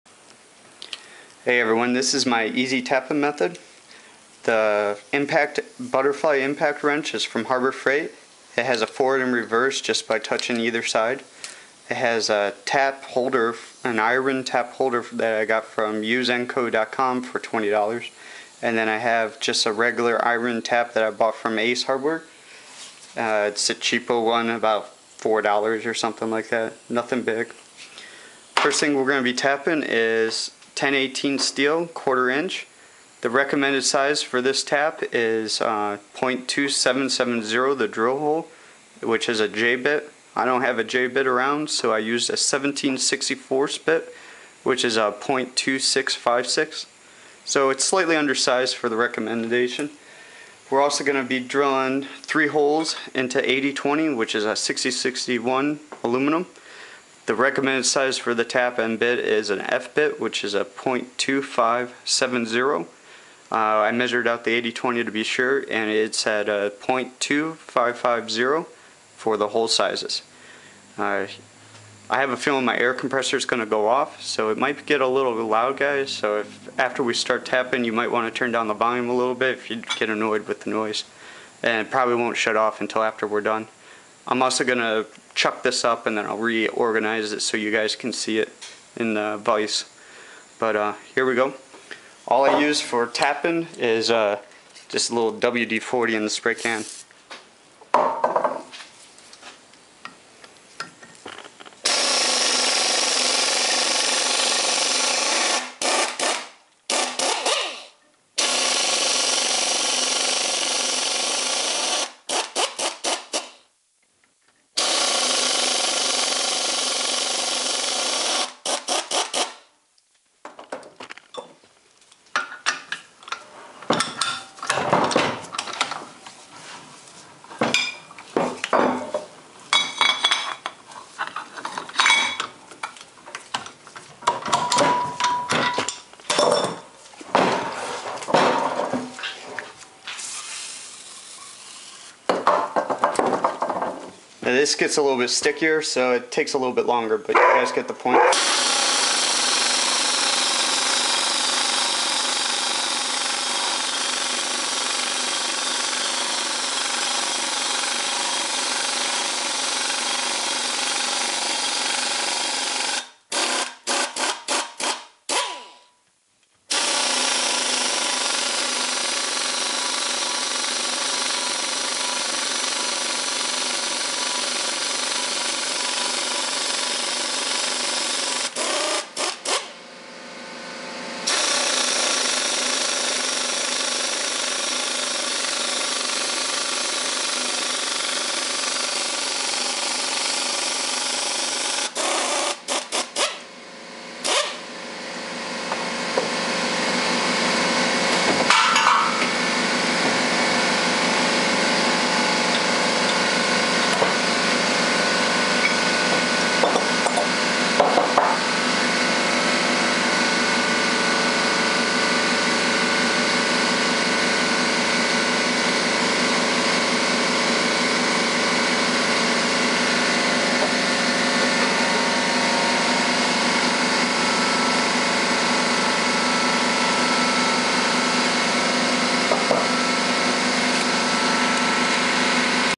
Easy Tapping Method
I learned about this new tapping method over on CNCcookbook.com I decided to give it a try and impress with the results.  It takes about 3 seconds to tap 1/4 1018 steel and 15 seconds to tap 1 1/4" inches into 6061. Enjoy the video.